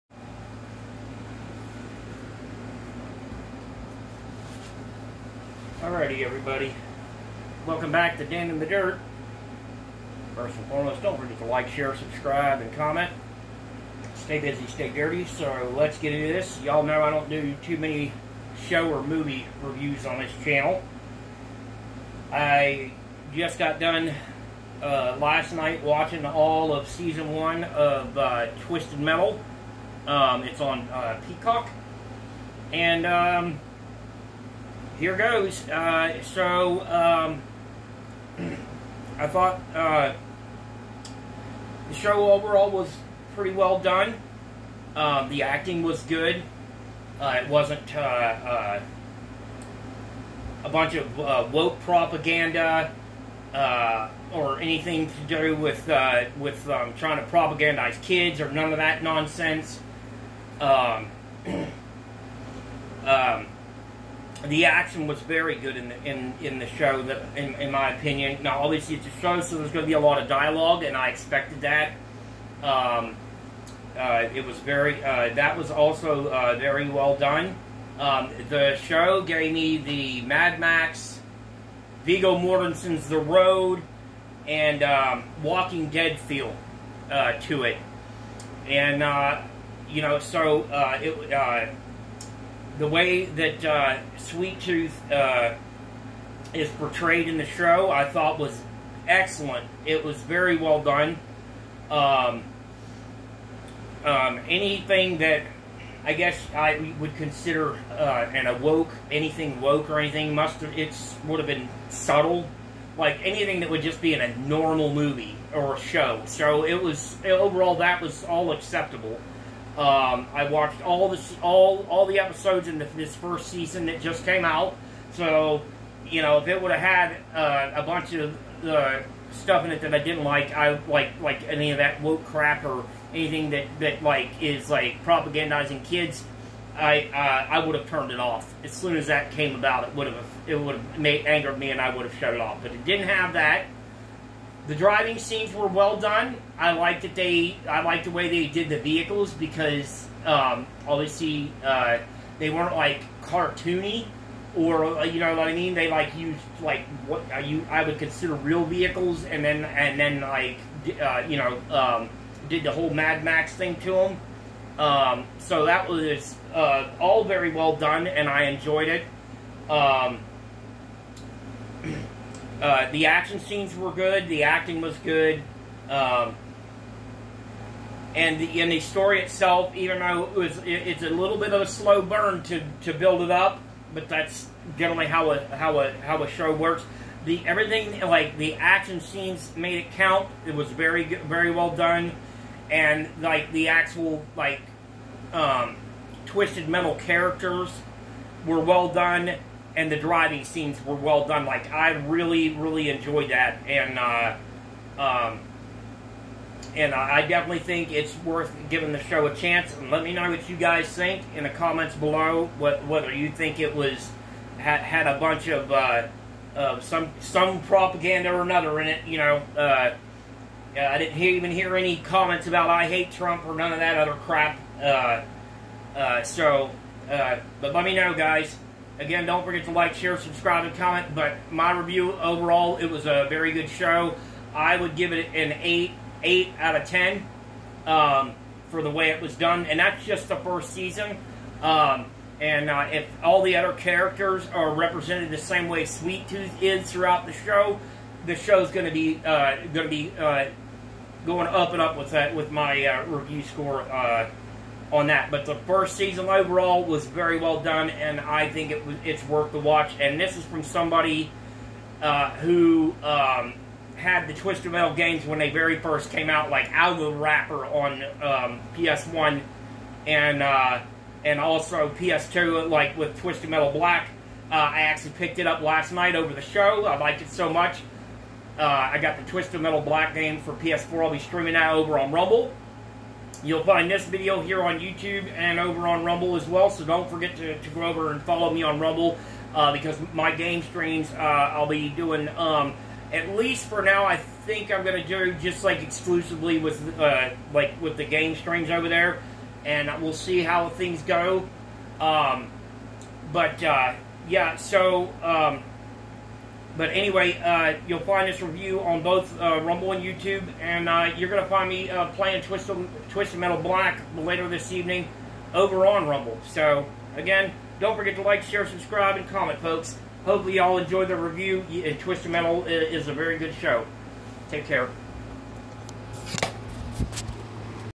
Twisted Metal TV Series Review Season 1
I discuss the new Peacock TV show Twisted Metal